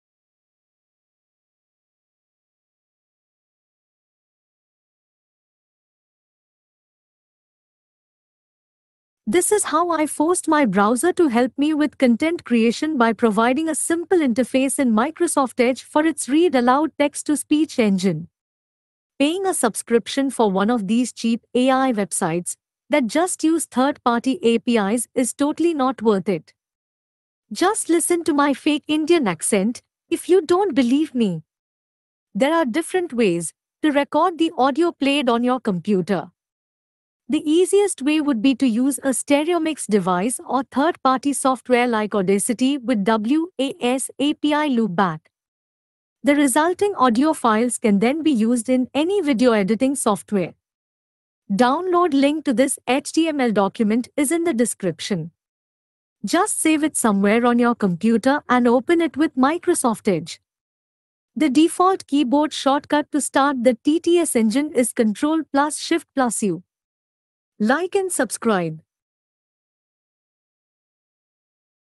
Using MS Edge TTS-Engine for content creation!
A very simple html page I wrote that helps me with content creating by leveraging MS Edge TTS-Engine.